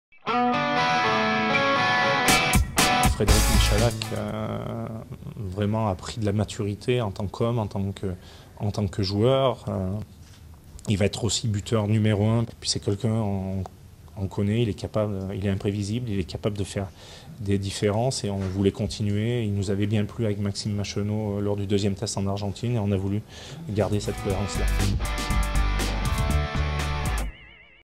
P. Saint-André : "Michalak est imprévisible"
Philippe Saint-André, le sélectionneur du XV de France a évoqué les raisons pour lesquels il a décidé de faire confiance à Frédéric Michalak